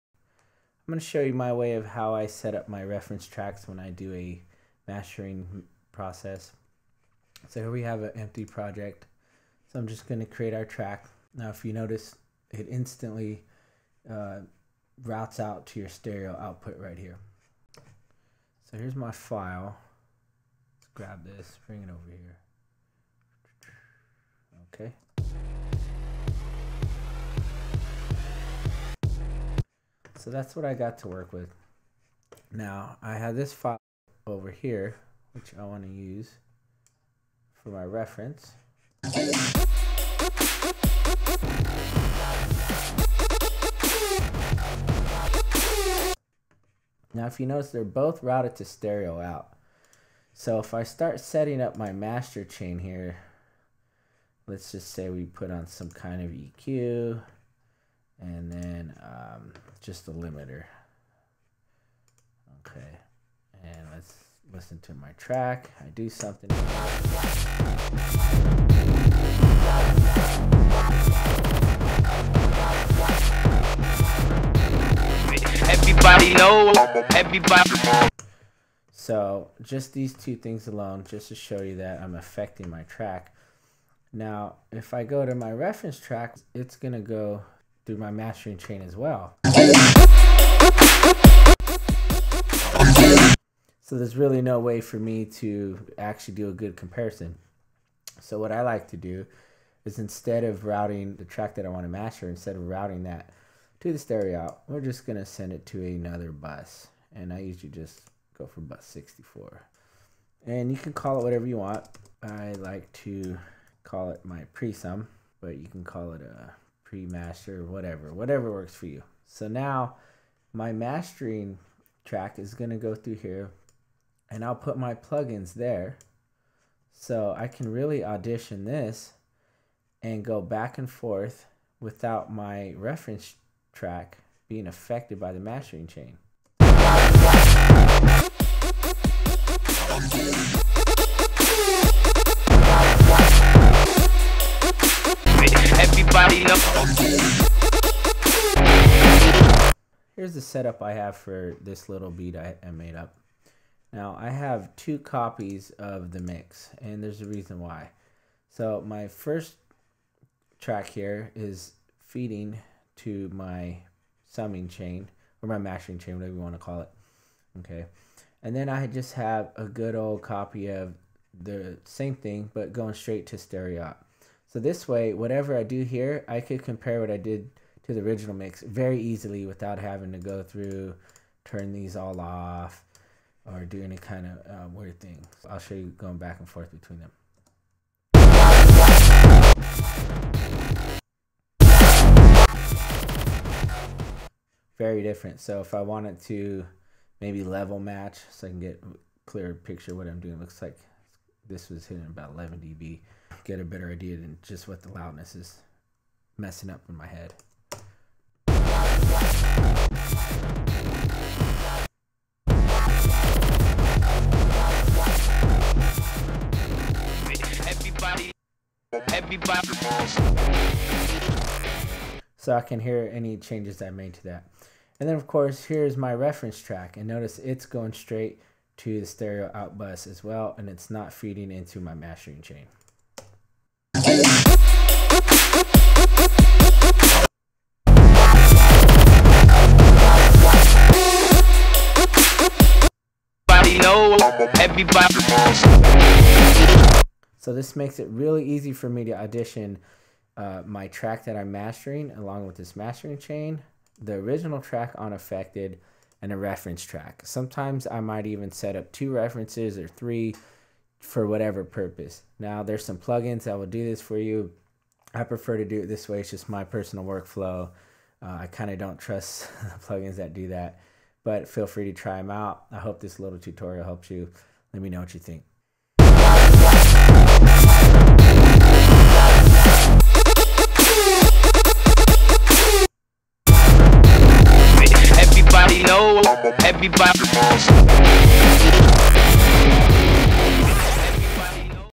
How to Set Up Reference Tracks on Logic Pro X
Here's my workflow when setting up reference tracks in a mastering session inside Logic Pro X.

TL;DR version:

1. On Mastering track Change 'Output' from 'St Out' to any open 'Bus #'. I prefer to use Bus 64.
2. On Reference track keep 'Output' to 'St Out'. This is usually selected by default.
3. Do not put ANY plug-ins on 'Stereo Out' channel with the exception of a meter. This will make sure that your reference track isn't being changed by any effect processing.

That's it! You can set up multiple reference tracks this way (as I've done in the video) as well.The reason I don't add processing to the mastering track channel is so that I have the option to send the signal out for parallel processing WITHOUT going through the mastering chain, this can be very useful.